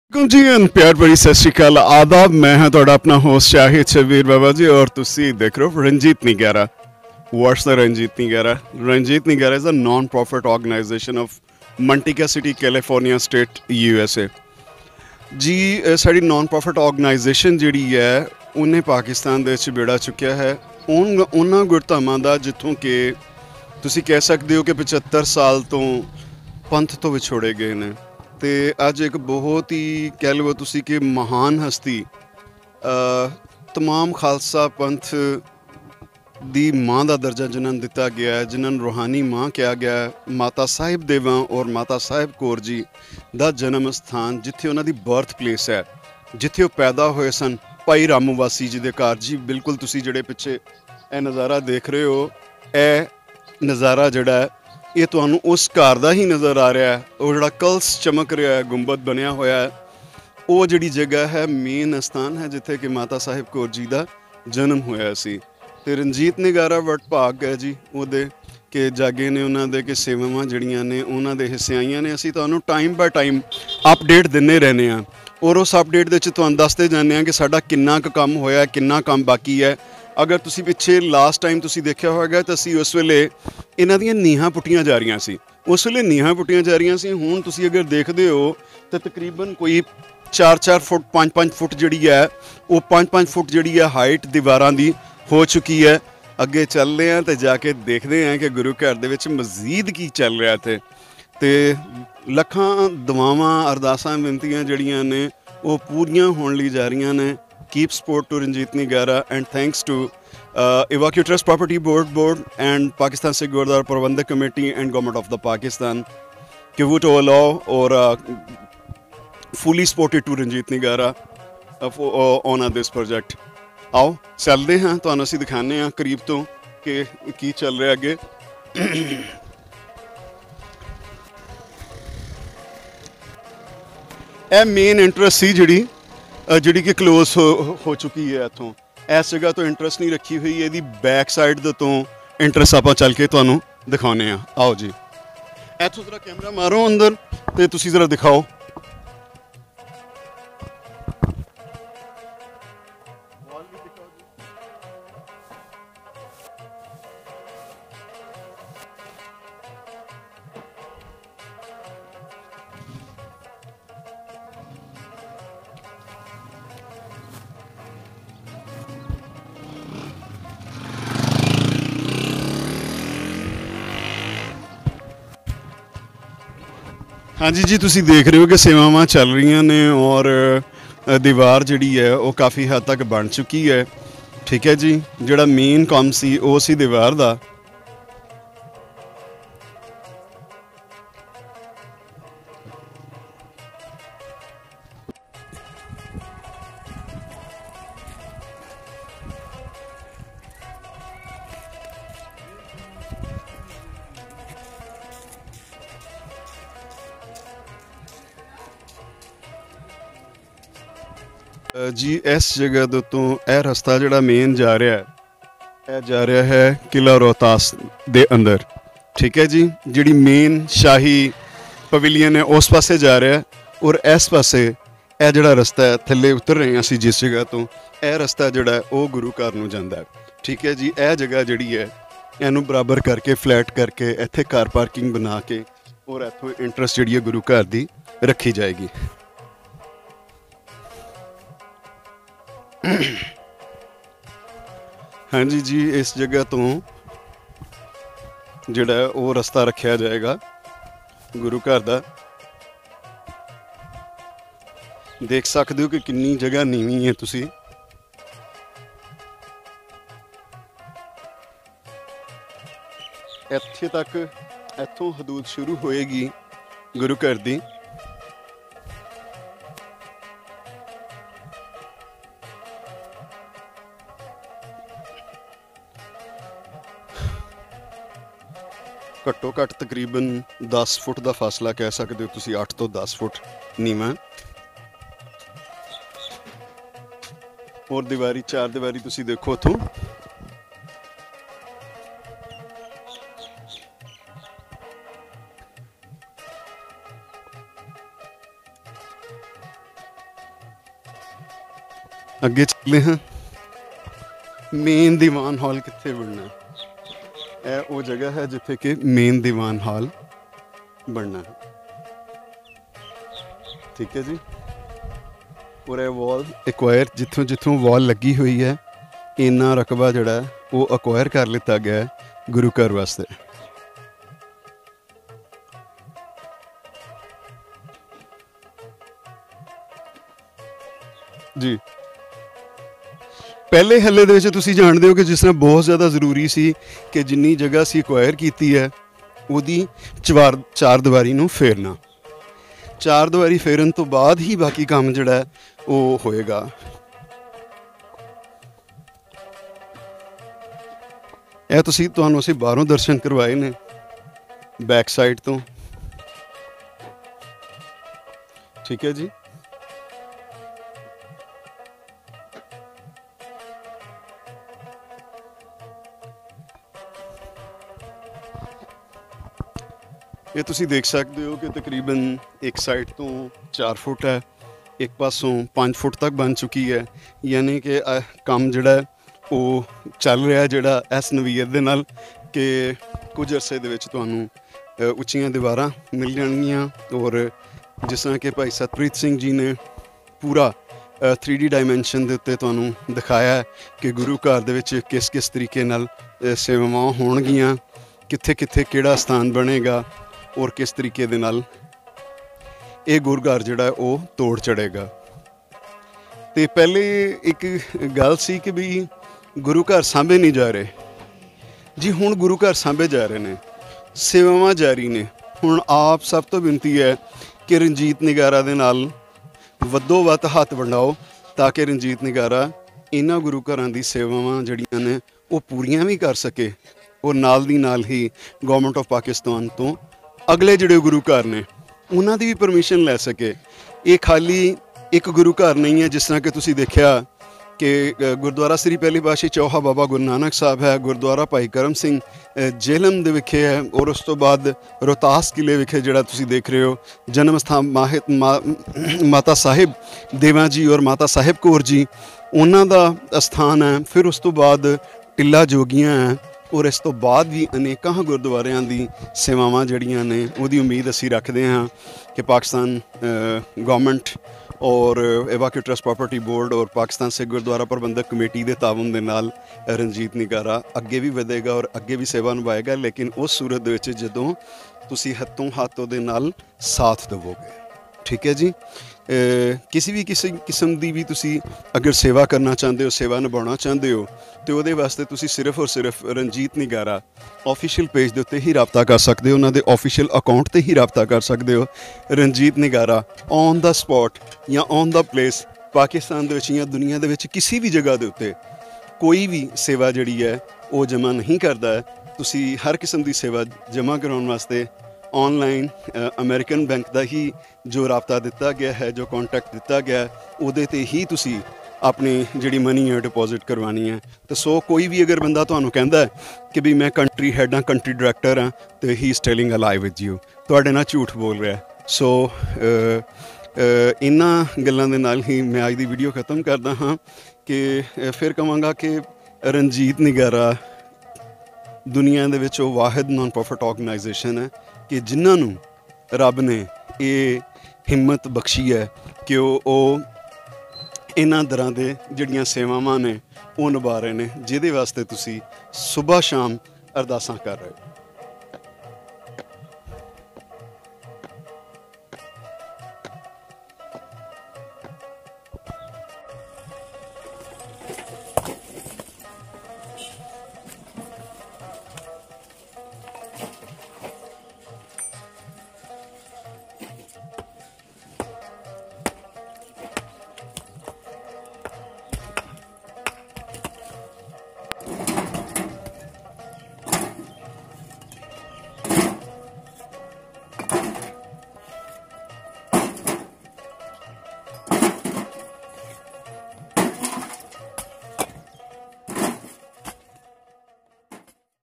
Latest update of Janam Asthan Mata Sahib Kaur ji
Latest update of Janam Asthan Mata Sahib Kaur ji inside of Rohtas Fort Tehsiel Dina District Jhelum North West Punjab Pakistan by Ranjit Nagara.